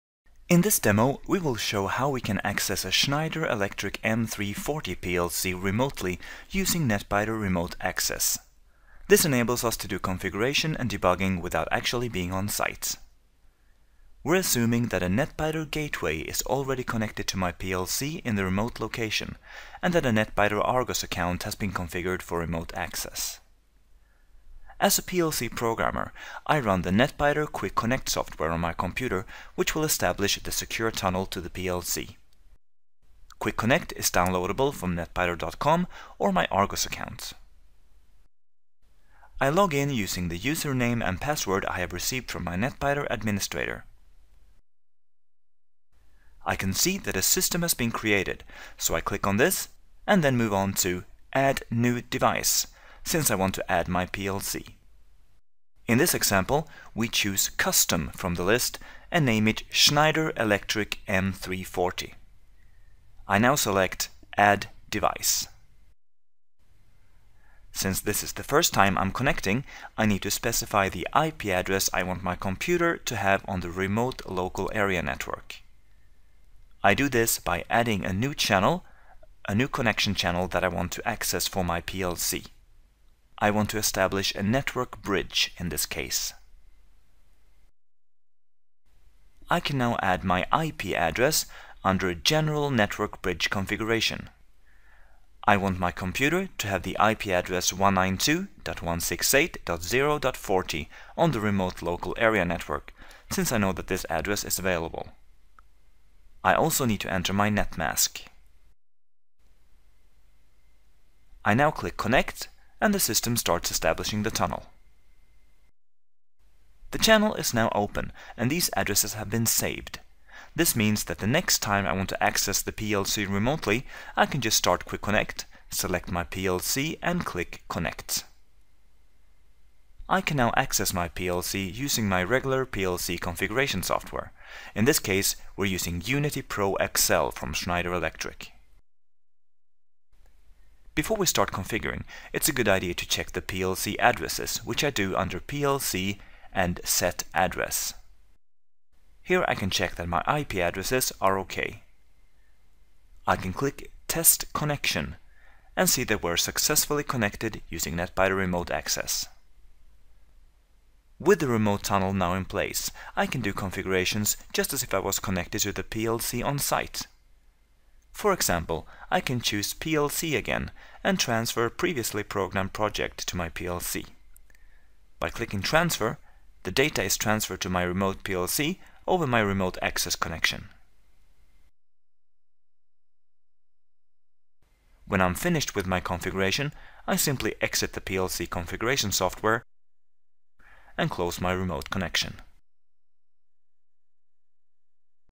How to configure a Schneider Electric PLC remotely using Netbiter
Accessing a Schneider Electric M340 PLC using Netbiter Remote Access.